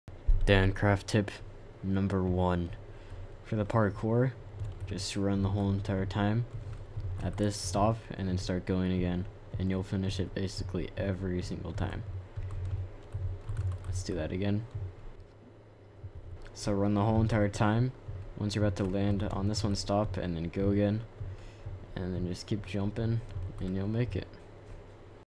How to Complete Dancraft Ulti Parkour (Dancraft Tips And Tricks) - Episode 1
In this episode I will be showing you how to complete the Dancraft Ulti Parkour with no issues and as always be sure to leave some support on the video, thanks!

Server IP: Slayerkey.Dancraft.Net